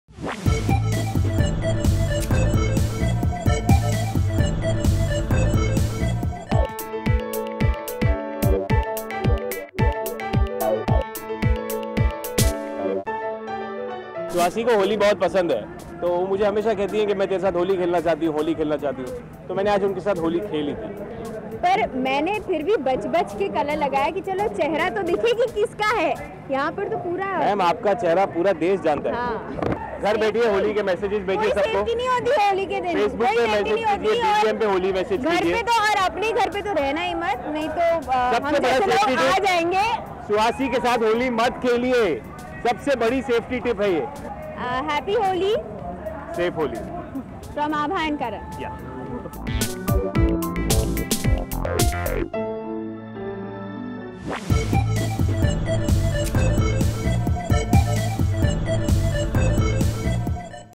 Happy Holi from television stars Karan Grover and Suhasi Dhami!!!
Watch television stars of "Yahan Mein Ghar Ghar Kheli", Karan Grover and Suhasi Dhami, wishing everyone a Happy Holi!!!